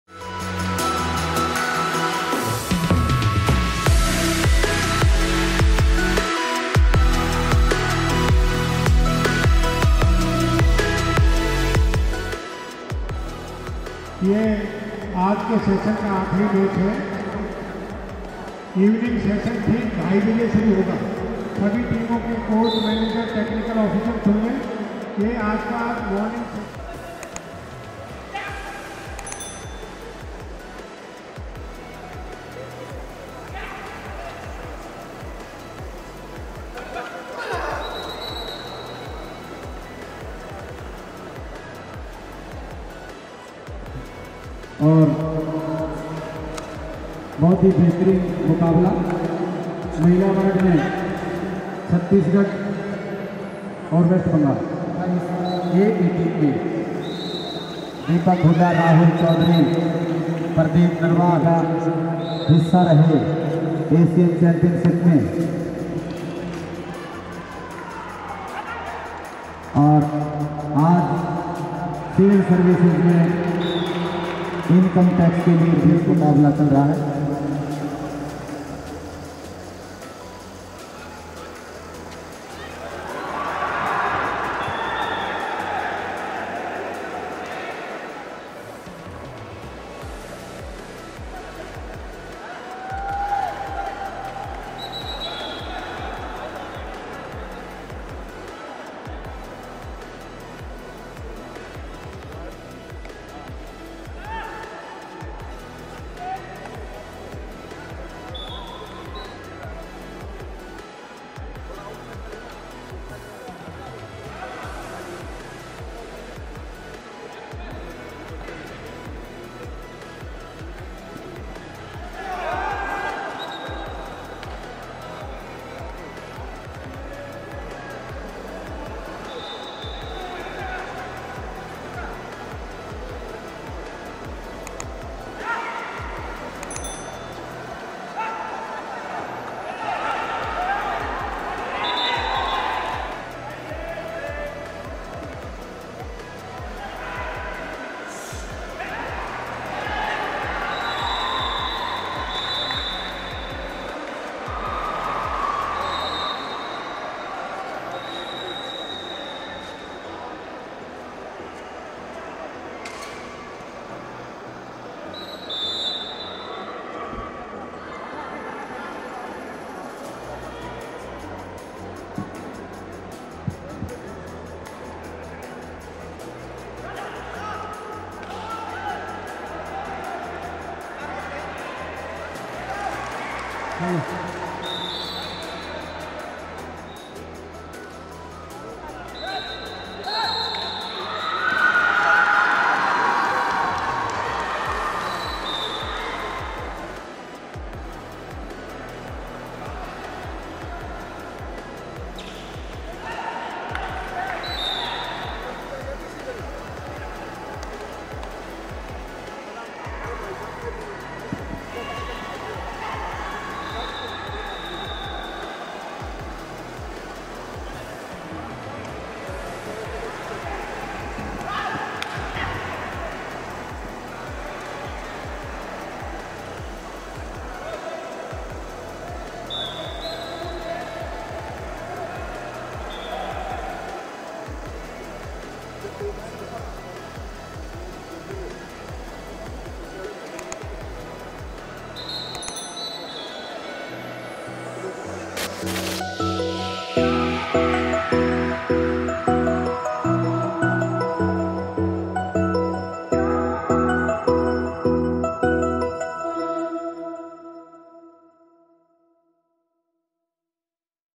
Rahul Chaudhari back in Form |All Raids & tackles in the All India Civil Services Kabaddi Tournament
‘Showman’ Rahul Chaudhary looked in fine form in the recently concluded All India Civil Services Kabaddi Tournament. Playing for CS Delhi, he teamed up with many star Kabaddi players including ‘Dubki King’ Pardeep Narwal, Mohit Chhillar, Rakesh Narwal. Rahul Chaudhary played exceptionally well in the tournament and scored many points. He showcased top class raiding abilities and also helped his team in the defence which led them to another title.